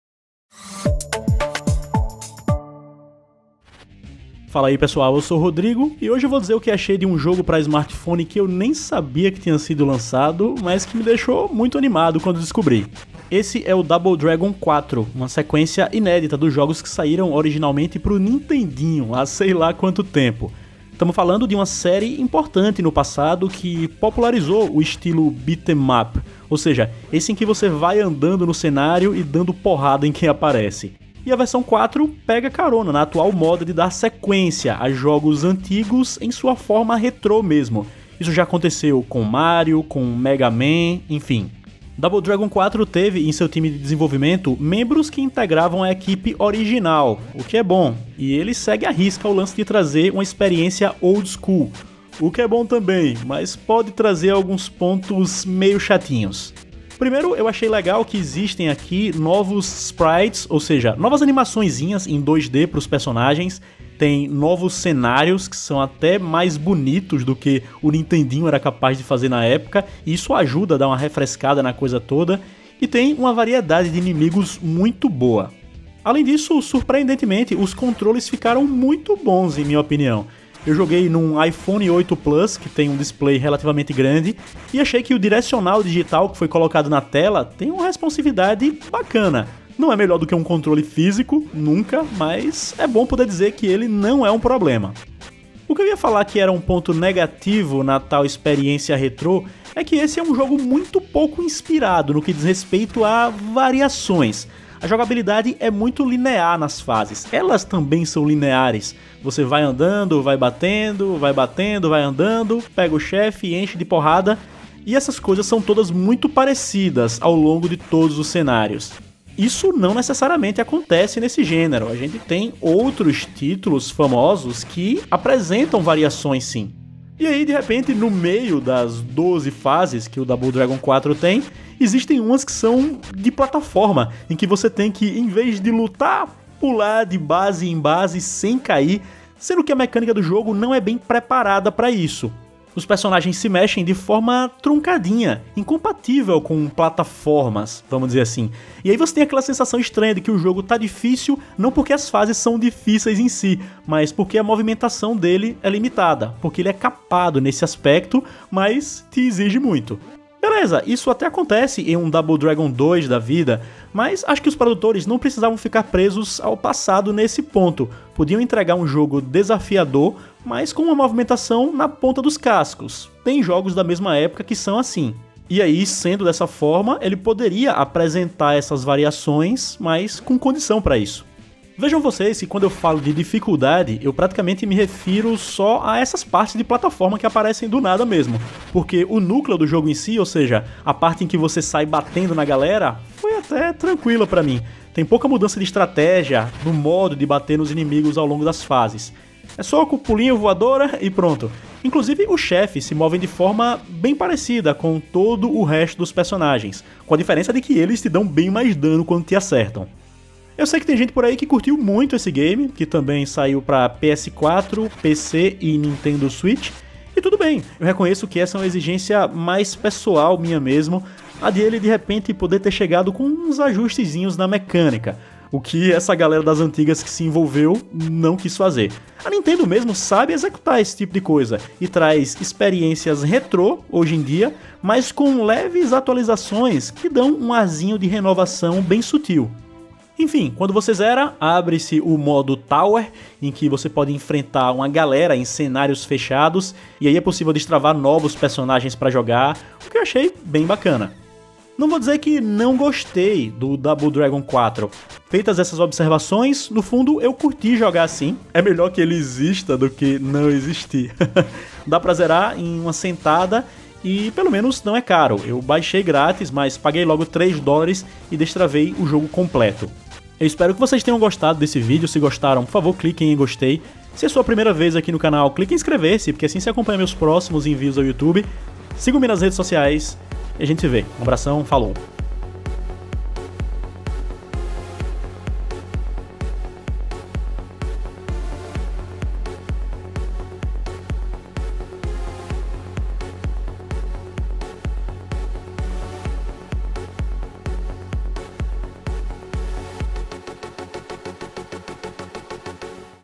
Double Dragon 4 (iOS) Análise
O que achei de Double Dragon 4, jogo retrô no estilo beat-'em-up para iOS (iPhone/iPod touch/iPad).